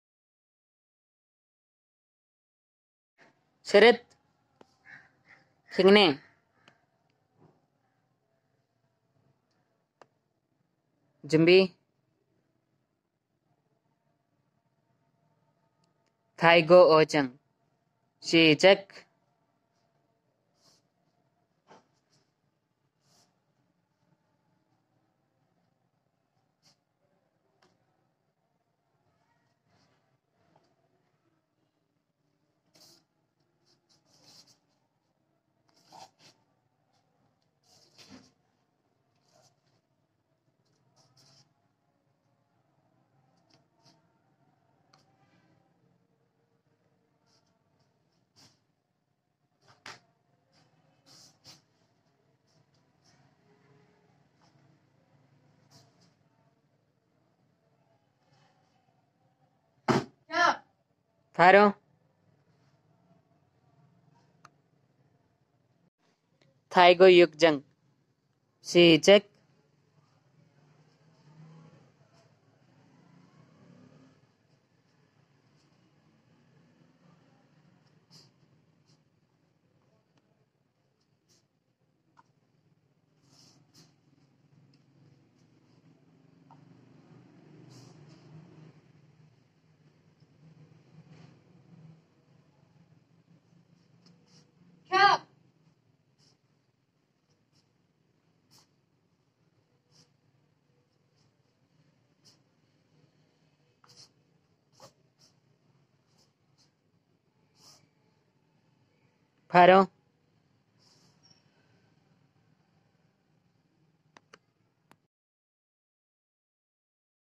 Pak Rec FU17 Bakhtawar Hur
2nd Sir Muhammad Babar Memorial - Open Online Taekwondo Poomsae Championship 2023 Pakistani Athlete Performance